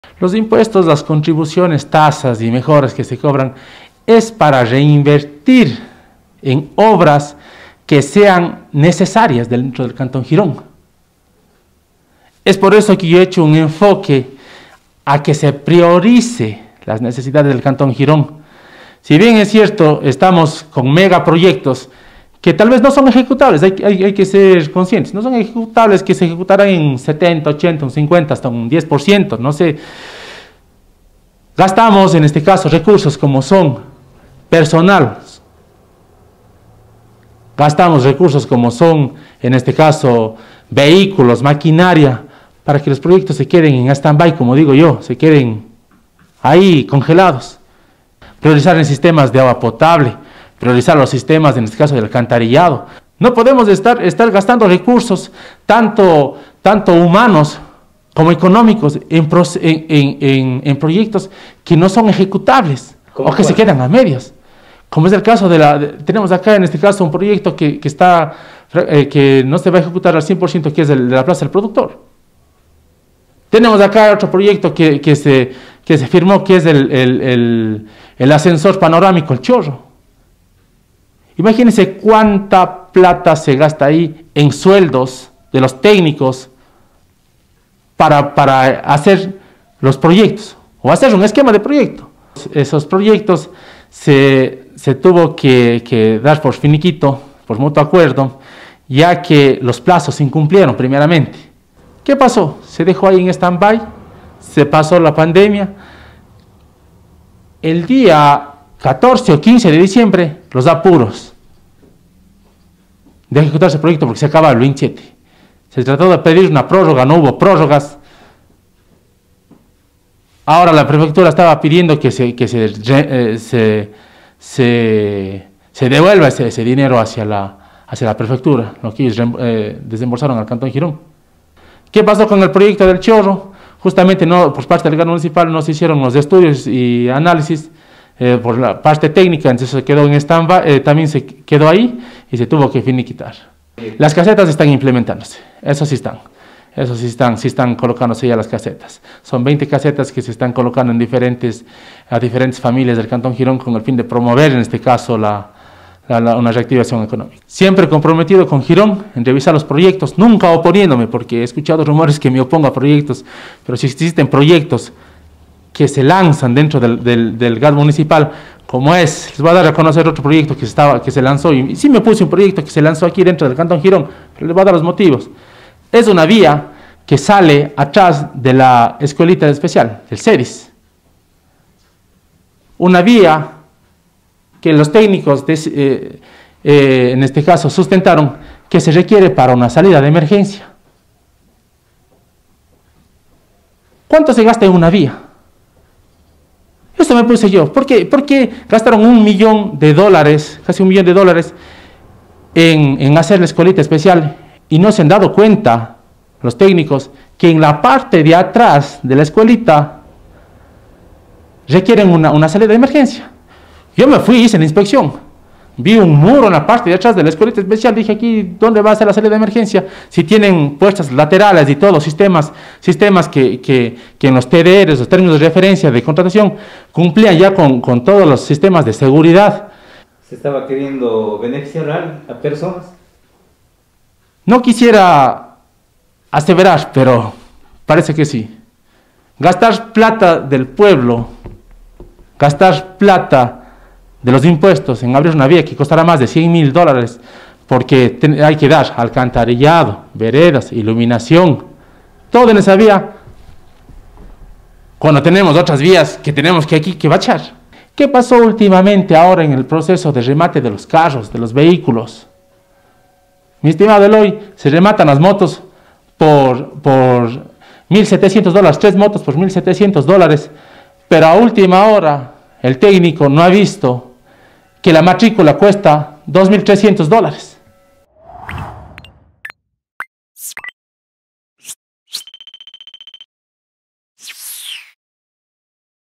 El concejal Javier Delgado denuncia proyectos que no se cumplen
Son varias semanas sin respuesta, como medio de comunicación hemos solicitado información concerniente a los proyectos firmados entre el municipio de Girón y la Prefectura de Azuay durante la administración de Yaku Pérez, tales como: Plaza del Productor, ascensor eléctrico en El Chorro, casetas en la vía; pero no hemos recibido respuesta alguna, según se manifestó estos proyectos estaban financiados pero nada se sabe. El concejal Javier Delgado se ha referido a este tema y otros proyectos municipales, los cuales a su decir, provocan erogaciones económicas innecesarias e impiden la ejecución de otras obras que urgen a la población.